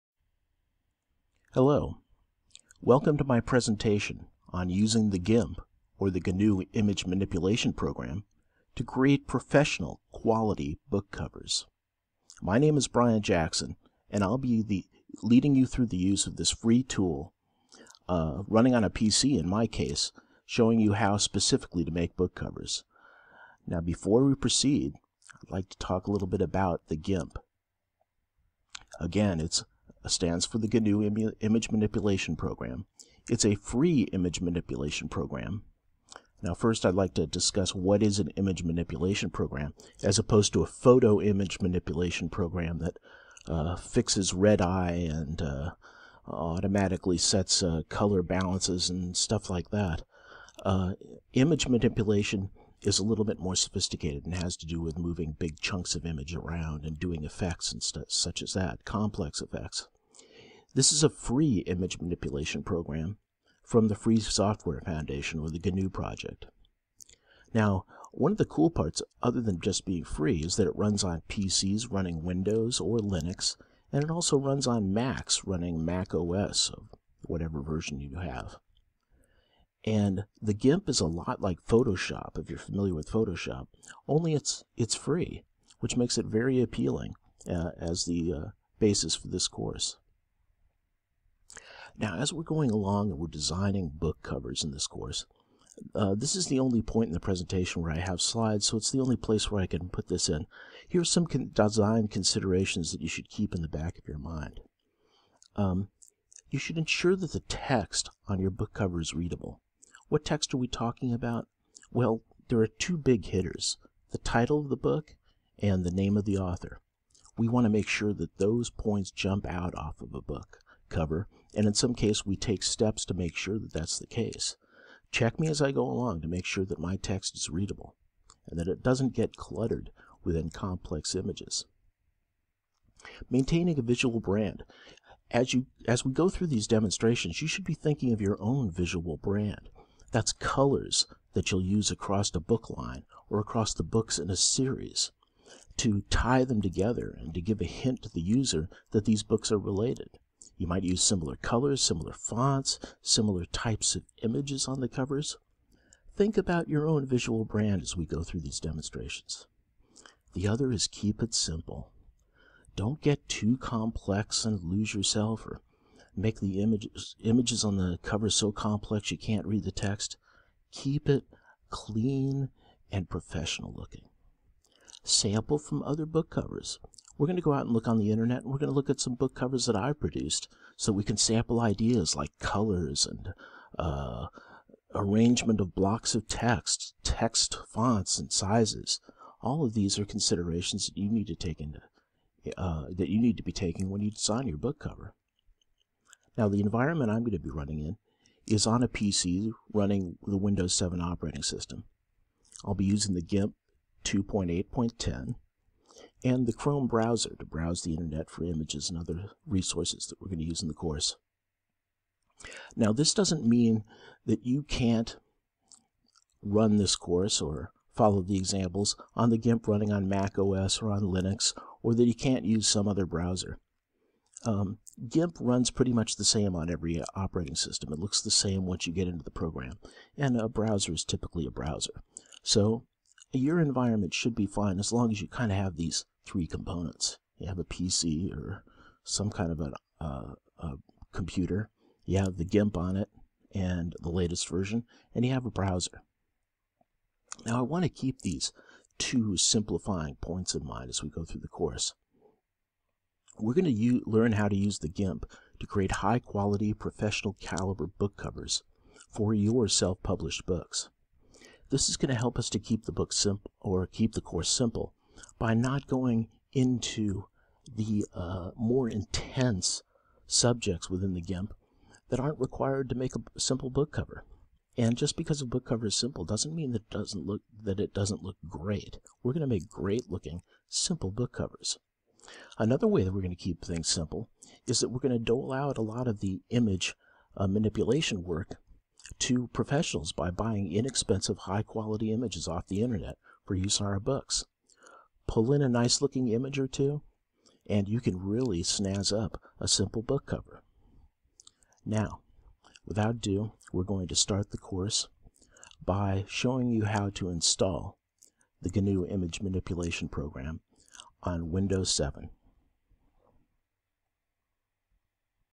GIMP for Beginners: Book Covers by Brian Jackson
"GIMP for Beginners: Book Covers" by Brian Jackson is a Udemy course that explains how to use the GIMP to create book covers for your self-published ebooks.

---Brian